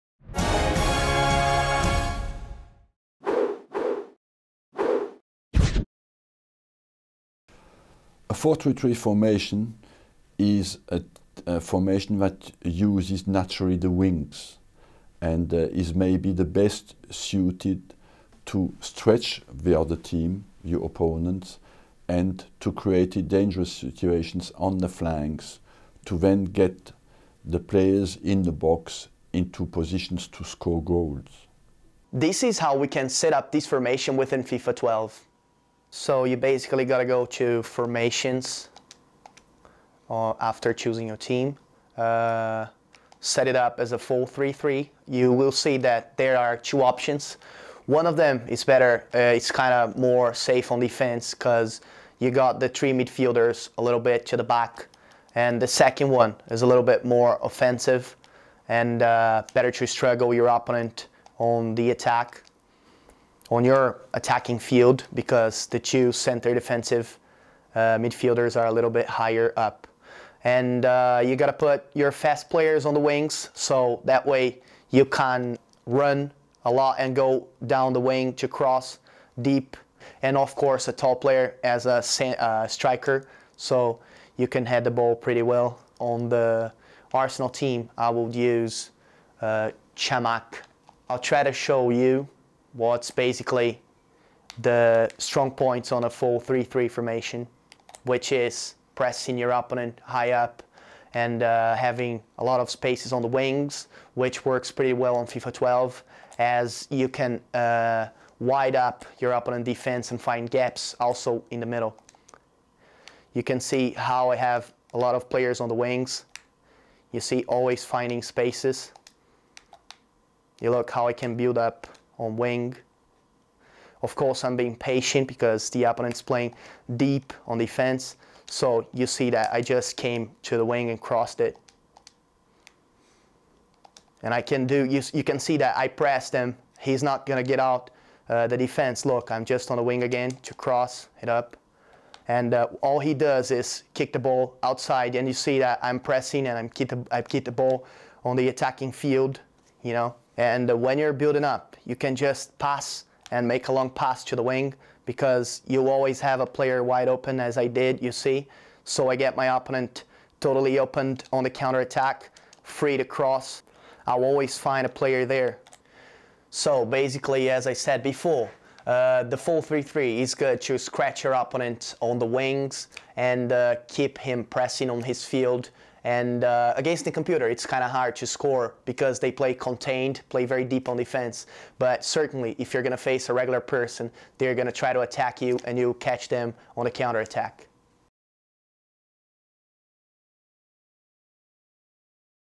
Wenger's tips: When to use 4-3-3
In this FIFA Interactive World Cup Training Camp iconic Arsenal manager Arsene Wenger explains when and how to use the 4-3-3 formation.